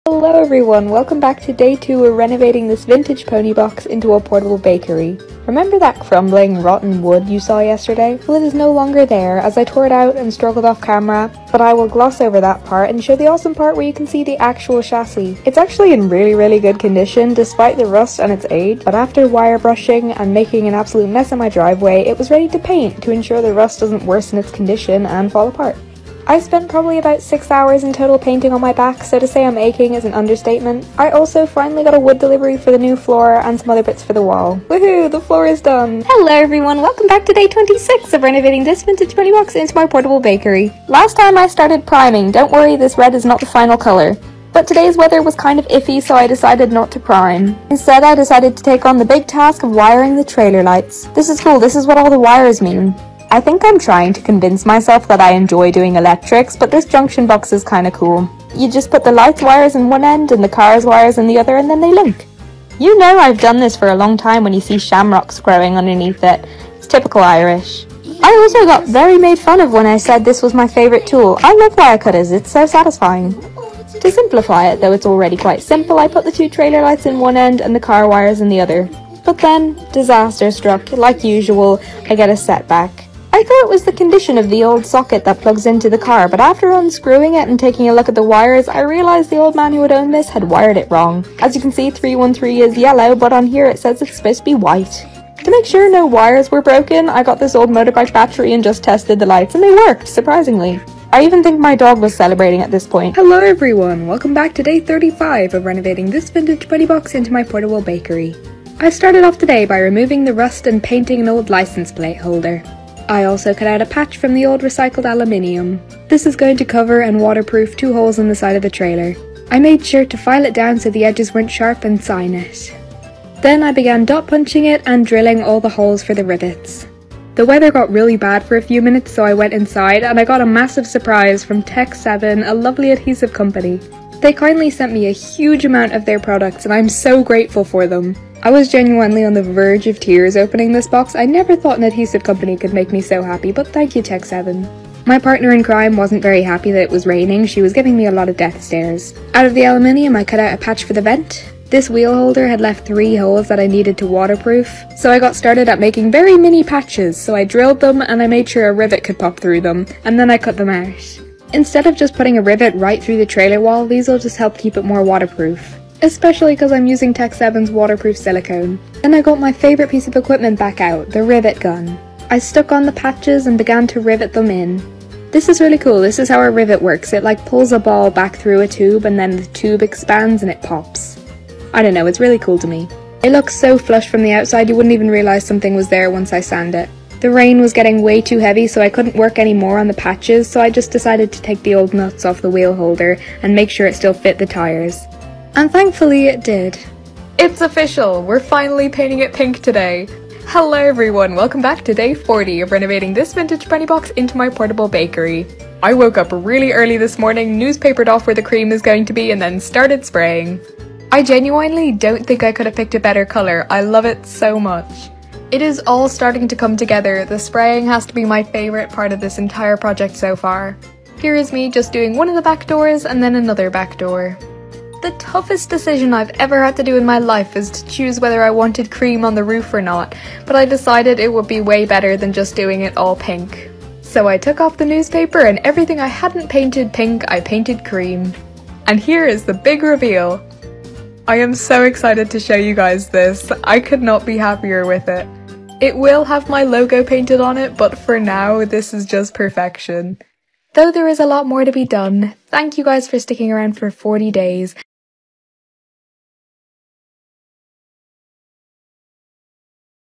Irish teenager transforms vintage pony box into a pop-up bakery stand | SWNS
An enterprising teenager spent five months converting a vintage pony box into a vehicle to house her business 'From Scratch Bakery'.

Alice Kelly, 18, spent the £512 (€590) she received for her 18th birthday on the pony box, which she found second-hand online.

The renovation was an ambitious project for the Alice, who had “barely used a hammer before”.